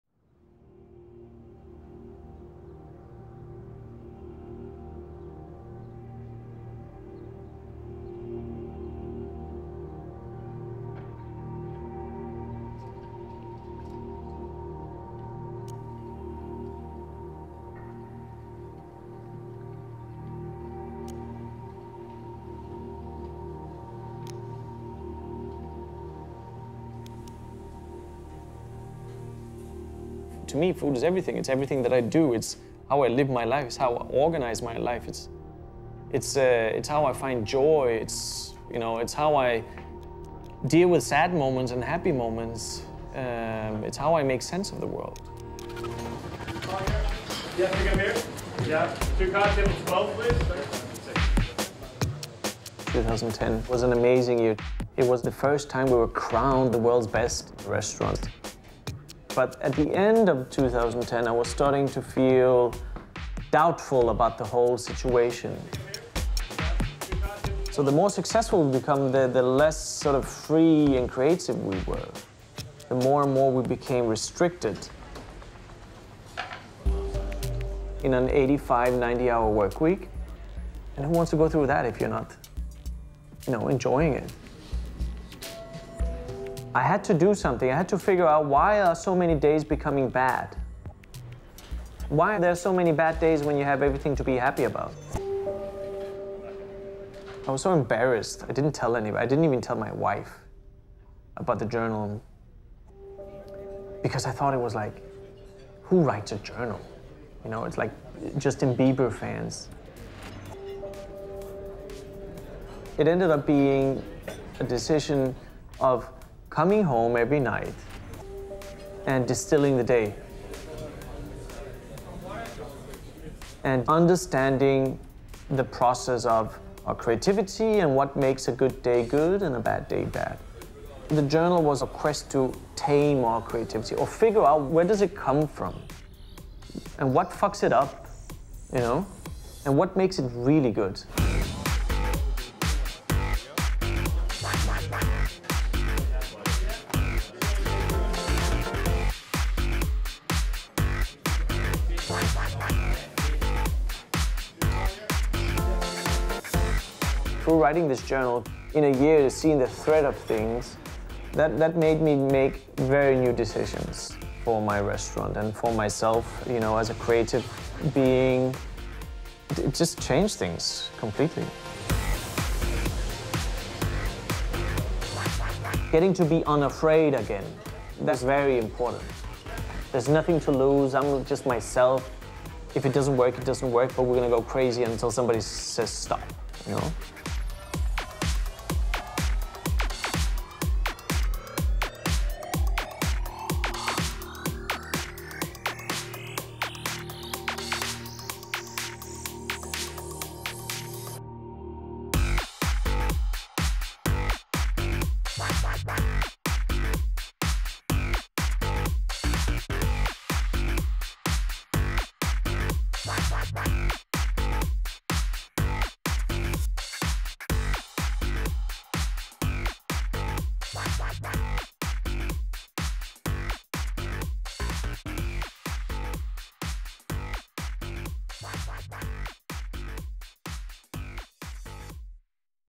René Redzepi: A Work in Progress
René Redzepi is chef/owner of Noma restaurant in Copenhagen. A four time winner of Restaurant Magazine's 50 Best Restaurant award, Redzepi took time out in 2011 to re-think success, creativity and how to better run his restaurant. In this film Redzepi discusses this period of reflection and the book that grew out of it - A Work in Progress.